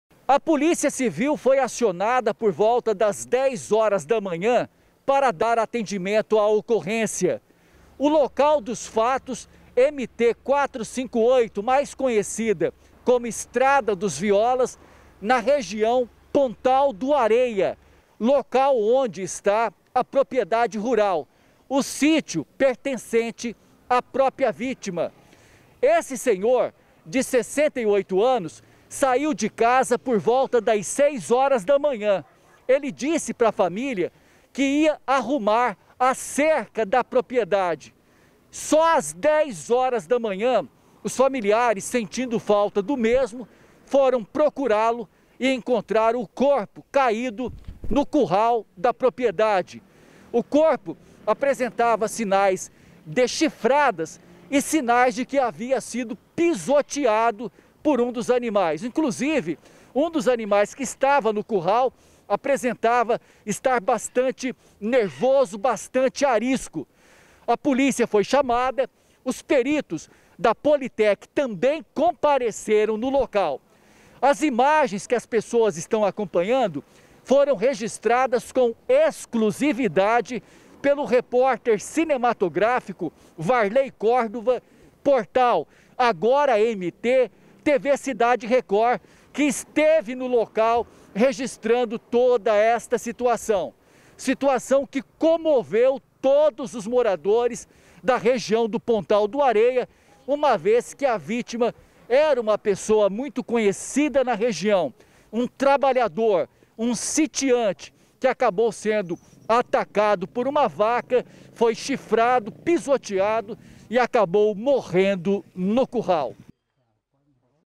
IDOSO DE 68 É ENCONTRADO MORTO EM CURRAL DE PROPRIEDADE
Um idoso de 68 anos perdeu a vida, após ser pisoteado por uma vaca, em uma propriedade rural de Rondonópolis. A Perícia Oficial e Identificação Técnica (Politec) esteve no local, analisando a cena da tragédia que abalou vizinhos da região. Confira.

Reportagem: Jandir Martins
Imagens: Varlei Cordova (Portal AGORAMT)